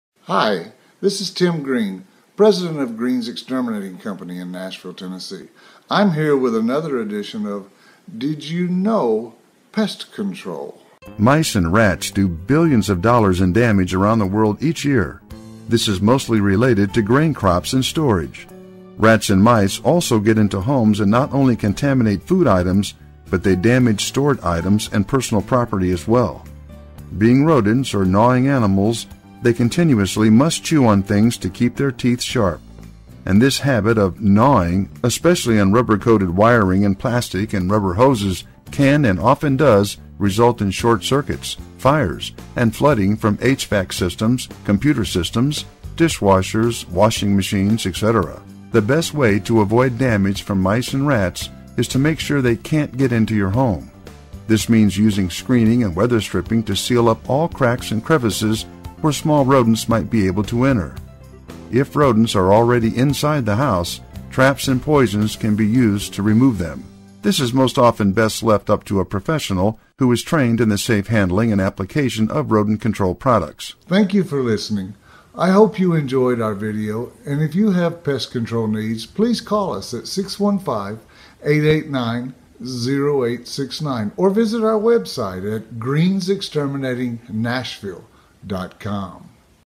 How to Get Rid of Mice in Nashville Tennessee
is your best solution to get rid of mice. In this video we explain your steps to a mice free Nasvhille TN home owners